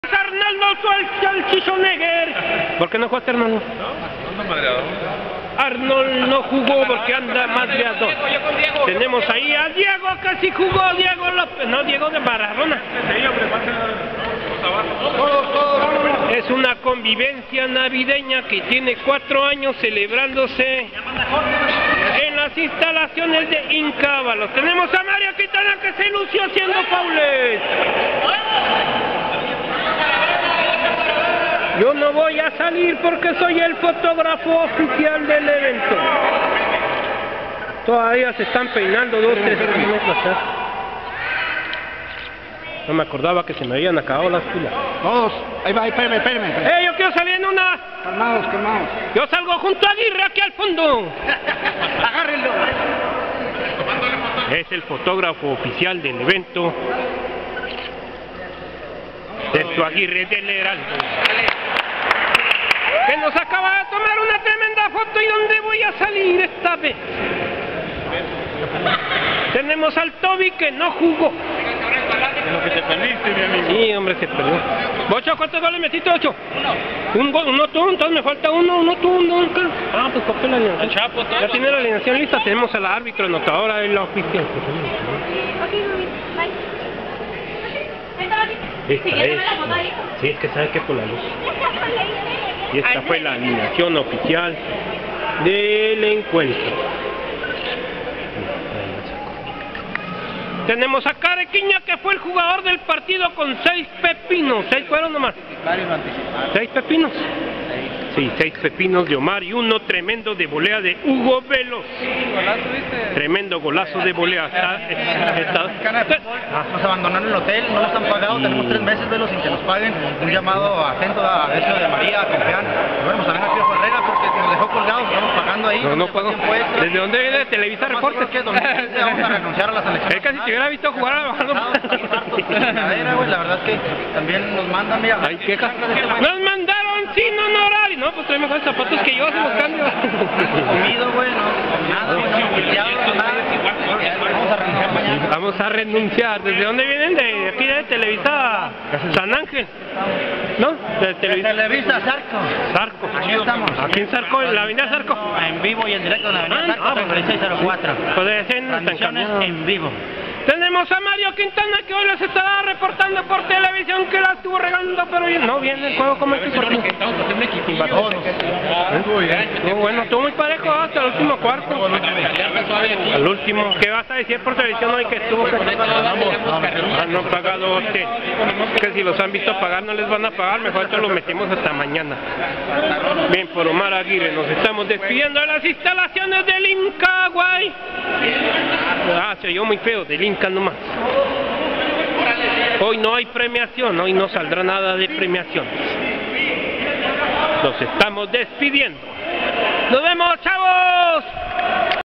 juego anual navideño convivencia Ymca 2014 despedida
parte final y despedida del evento navideño, cuadrangular conmemorativo recordando a los amigos eloy, lazaro y pedro omar aguirre en las instalaciones del ymca  chihuahua.